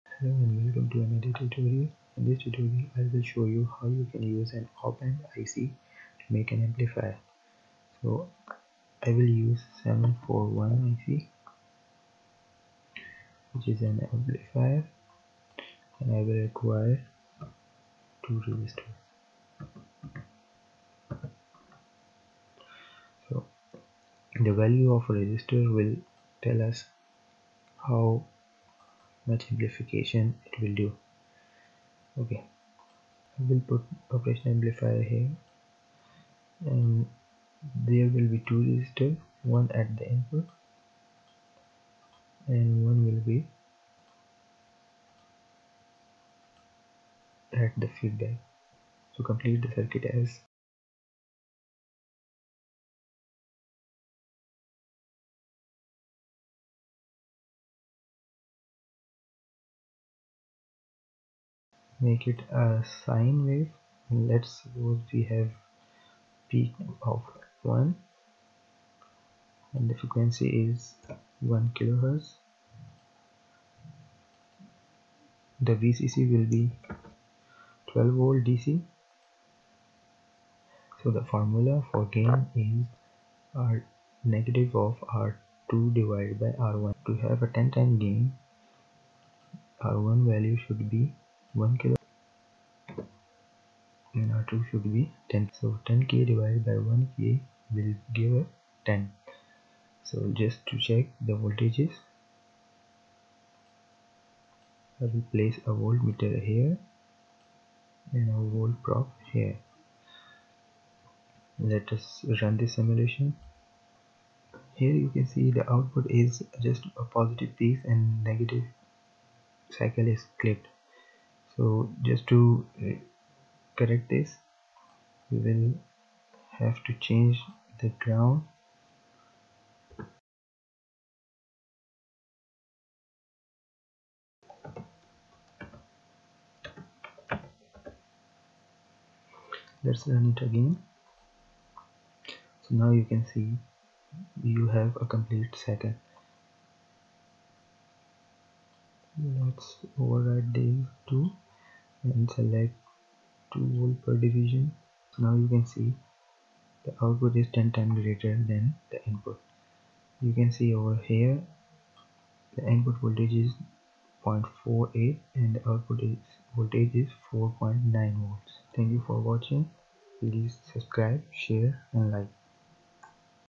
Amplifier designing using OP-AMP (741) in Proteus
This video is about designing an amplifier using 741IC in Proteus.

This Channel helps in teaching how to do different projects in Proteus, Matlab, Python and Shows different simulations of technical Projects.